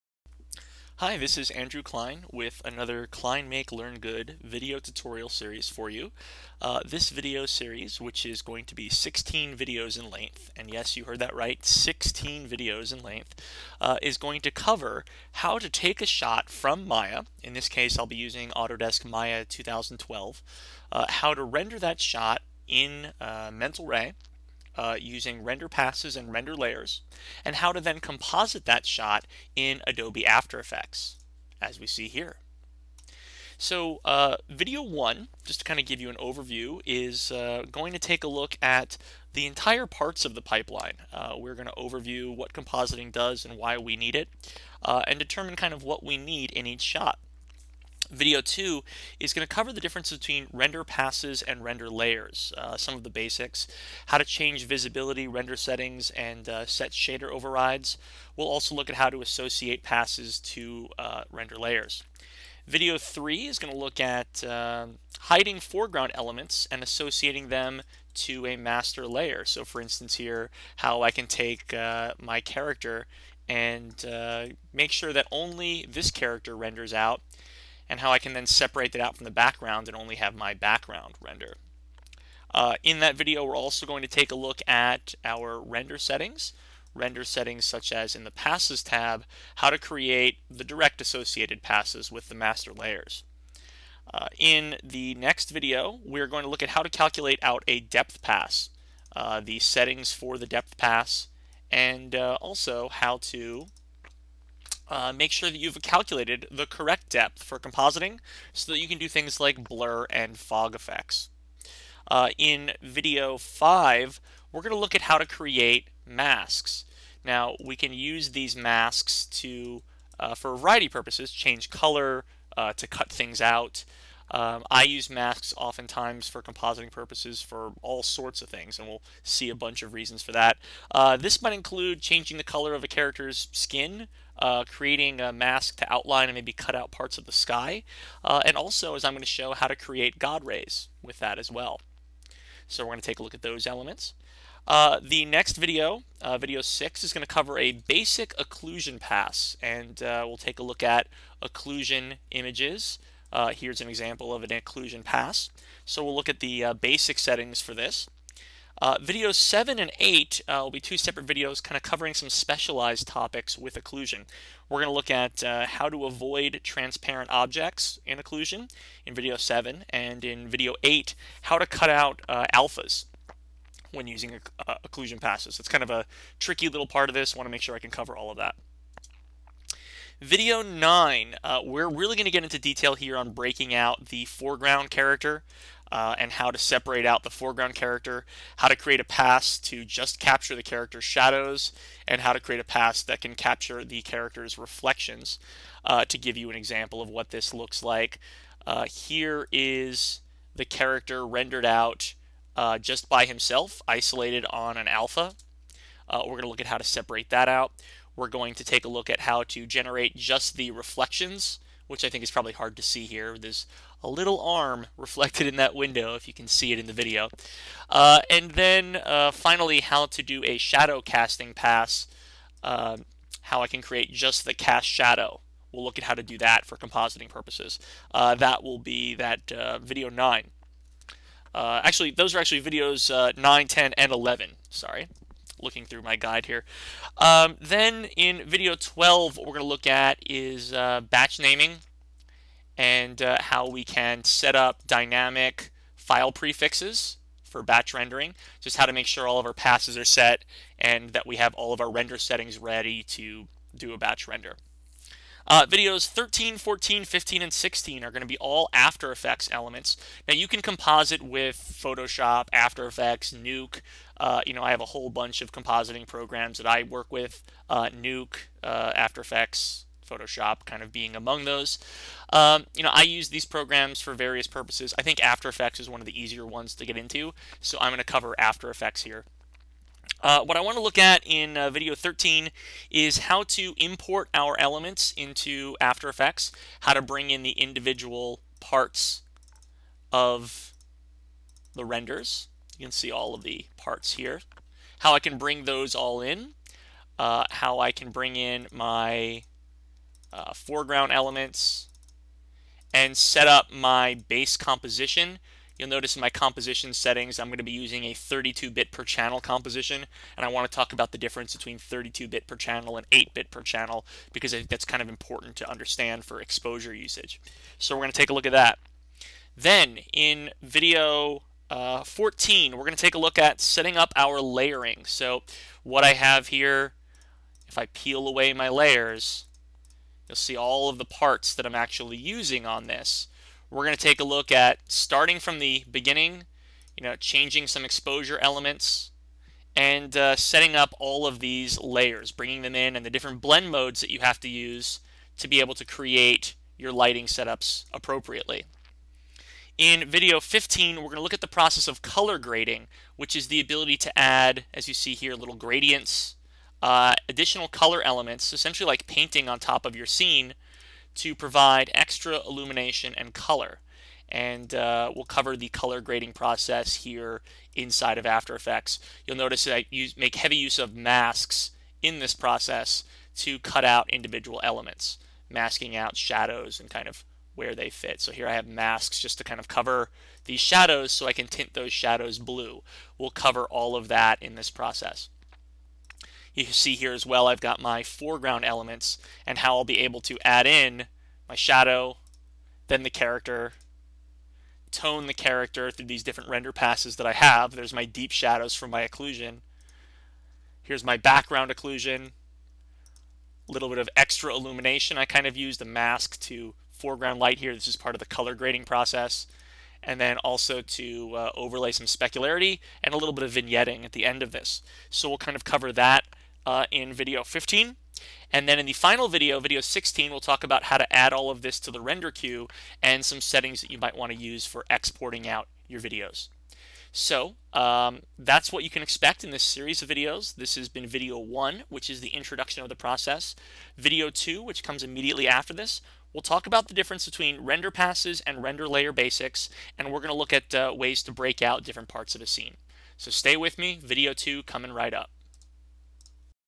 Maya-to-After Effects MultiPass Rendering Tutorial 1/16 by Andrew Klein
This video by Autodesk Certified Instructor for Maya Andrew Klein is an overview of the ideas behind multi-pass rendering and compositing, demonstrating different needs and presenting the foundations of the solutions that follow in the next 15 videos.